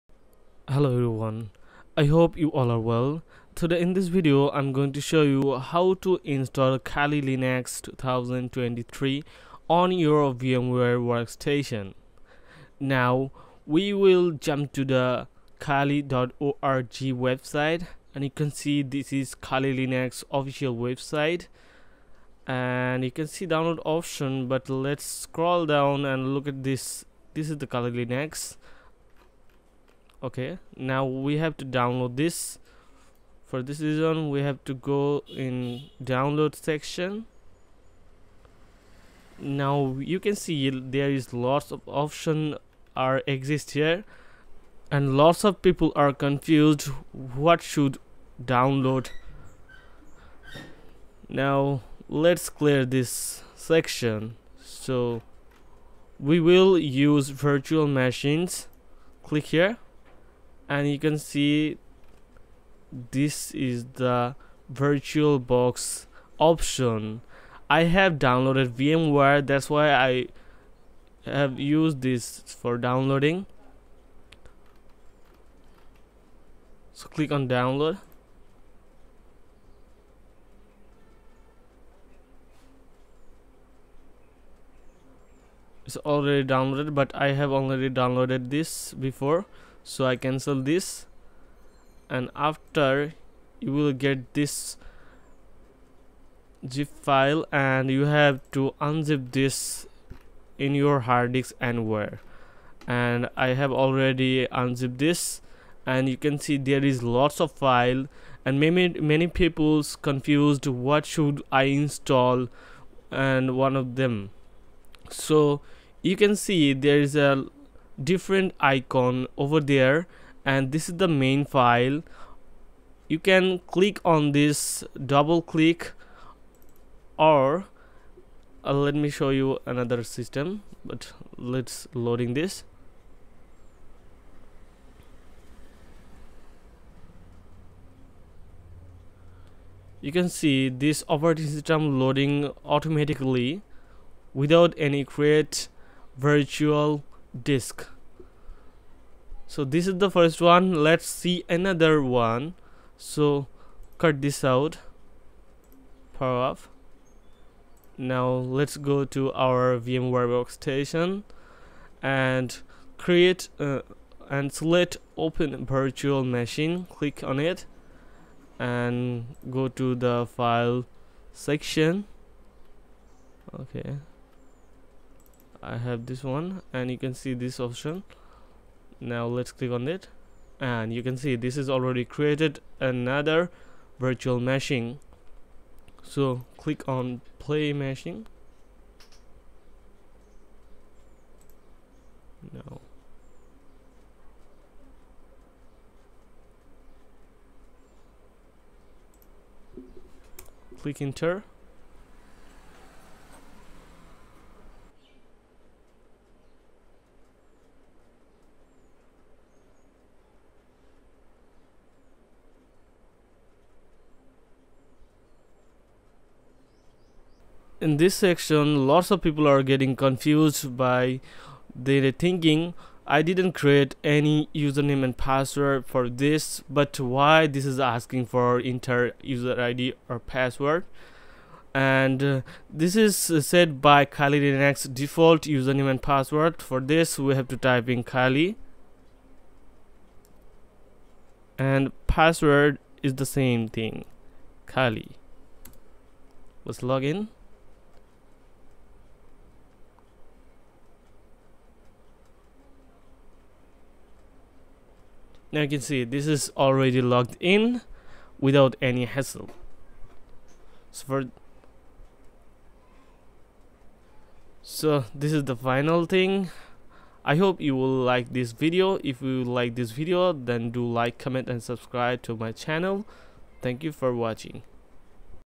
How to Install Kali Linux 2023 on VMware Workstation 17 Player
If you're interested in learning how to install Kali Linux 2023 on VMware Workstation 17, then this video is for you! In this comprehensive guide, we'll take you step-by-step through the installation process, making it easy for beginners to follow along.

Kali Linux is a popular operating system used by many cybersecurity professionals, as it comes with pre-installed tools that are specifically designed for penetration testing and ethical hacking. By running Kali Linux on VMware Workstation 17, you'll be able to create a virtual environment to safely test and practice your cybersecurity skills.

In this video, we'll cover everything from downloading and installing VMware Workstation 17, to setting up Kali Linux 2023 and configuring the virtual machine. We'll also provide helpful tips and tricks along the way to make the installation process as smooth as possible.

So, if you're ready to dive into the world of cybersecurity and learn how to install Kali Linux 2023 on VMware Workstation 17, be sure to watch this video!
Best Microphone
Best Keyboard
Best Mouse